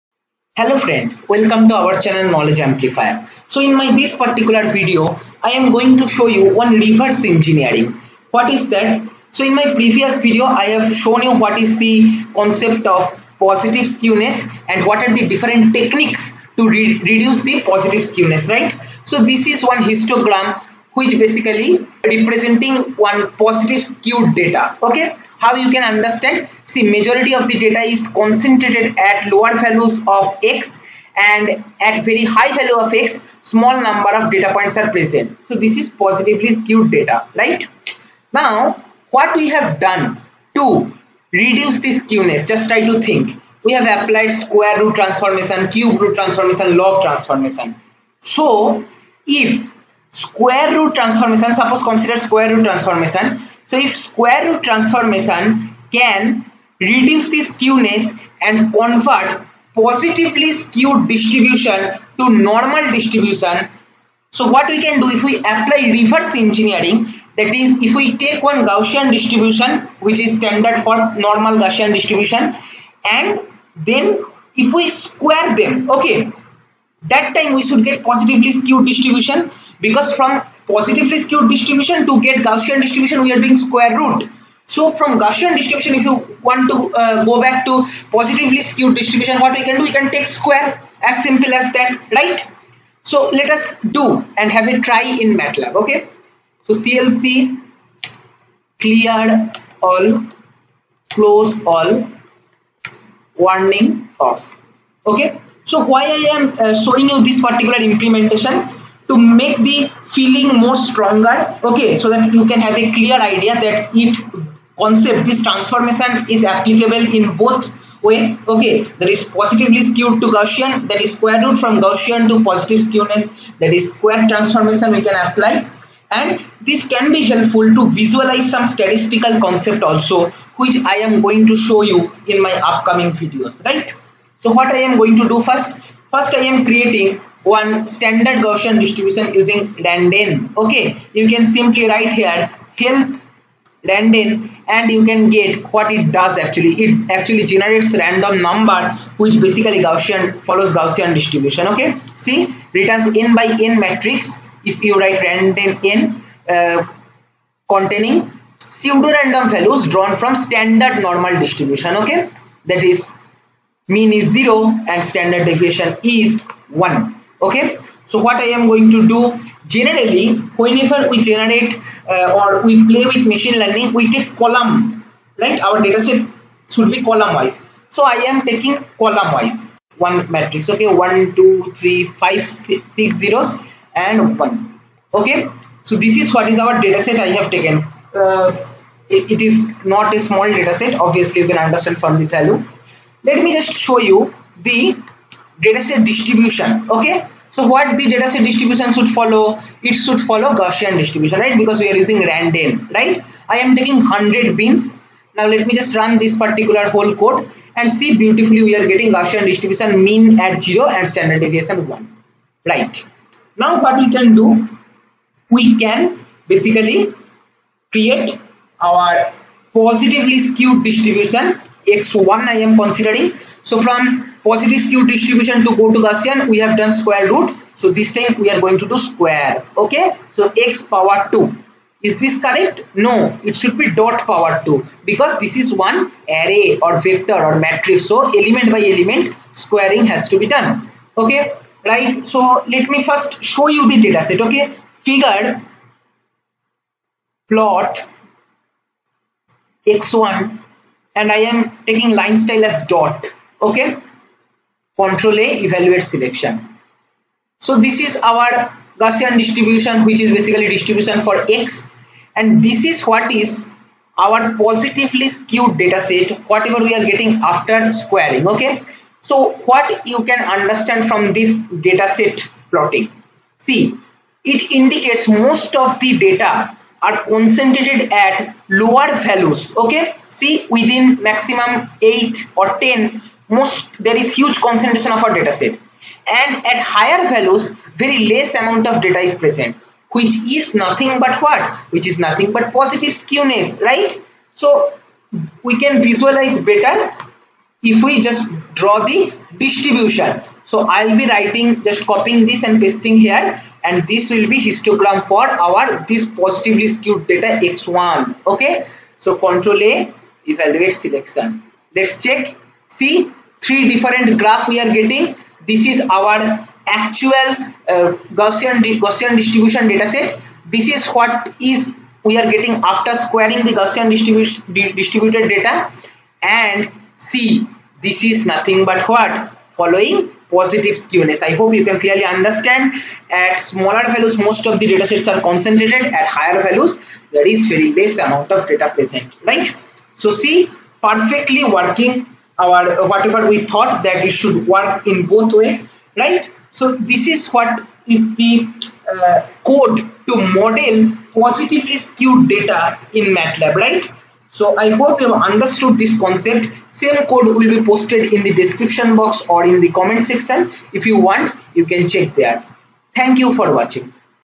How to generate Positively Skewed Data ?
Code:

clc
clear all
close all
warning off
x=randn(1000000,1);
hist(x,100);
title('Gaussian Distribution');
x1=x.^2;
figure;
plot(x1,'.');
title('Positively Skewed Dataset');
figure;
hist(x1,100);
title('Positively Skewed Data Distribution');

YOU JUST NEED TO DO